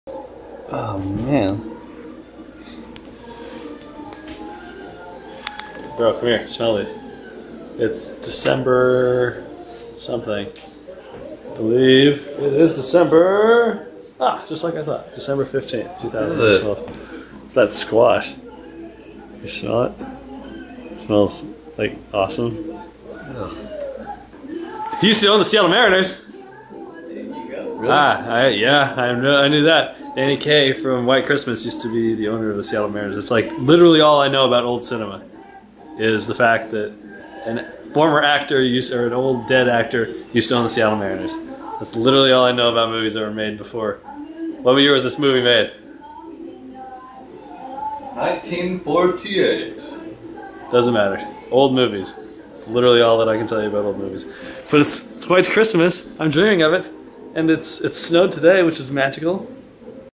Squash - Day 1080
December 15th. Dad baked a squash.